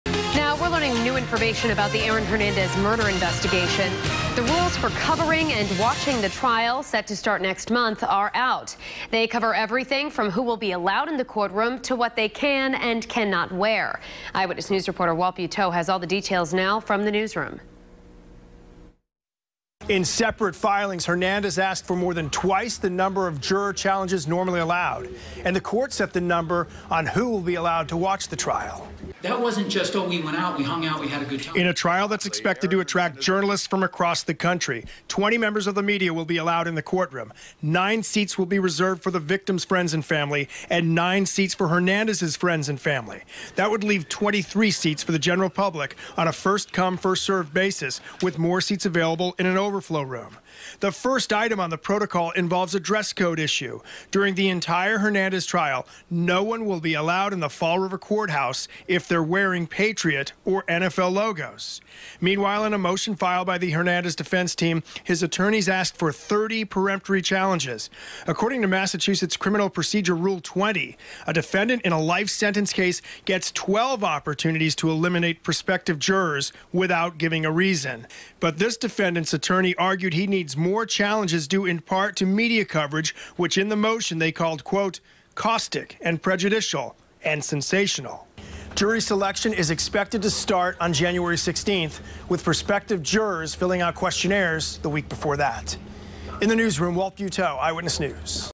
Rules of Aaron Hernandez trial released
They cover everything from who will be allowed in the courtroom, to what they can and cannot wear.